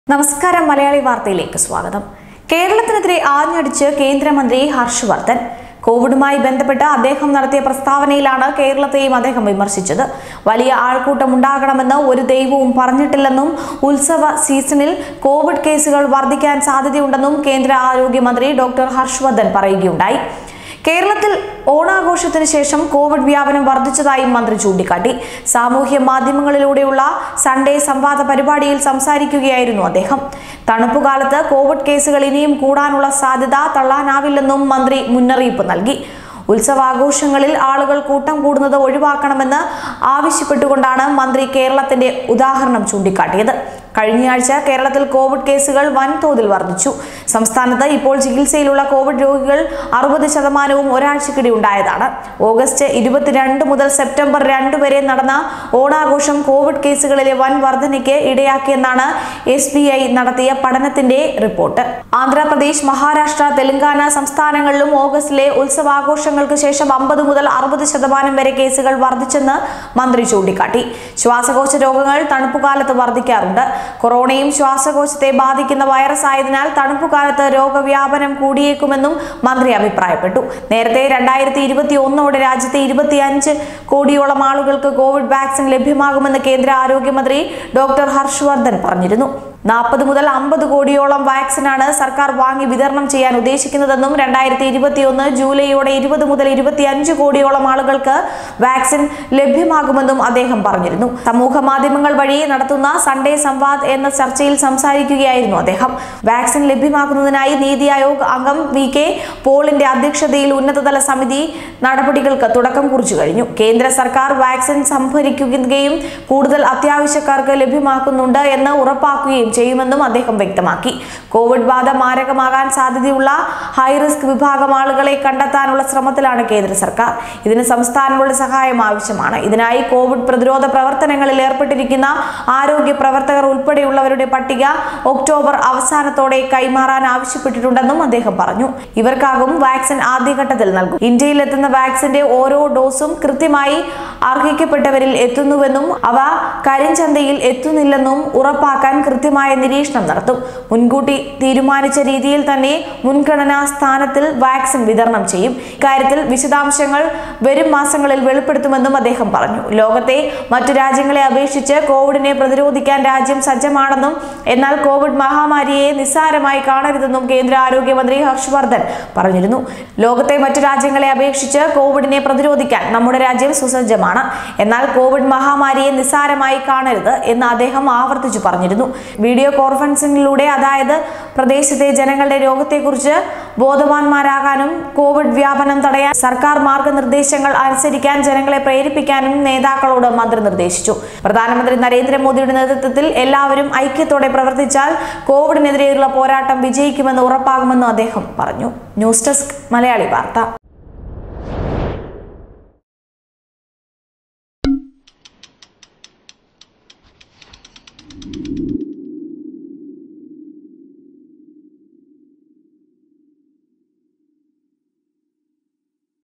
COVID: നിസ്സാരമായി കാണരുത്
ഏത് ദൈവമാണ് അങ്ങനെ പറഞ്ഞത്?
കേരളത്തിനെതിരെ ആഞ്ഞടിച്ച് കേന്ദ്രമന്ത്രി